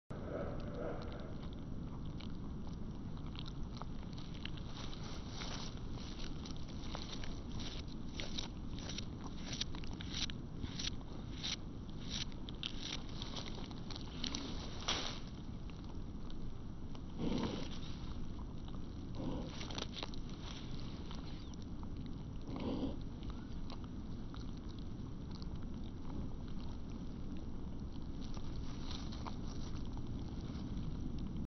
Hedgehog scratching in slow motion
Hedgehog scratching in slow motion. I managed to convert original 120fps HD Sony FDR-X1000V video to 25% speed slow motion with Adobe premiere. Now it shows better what happens.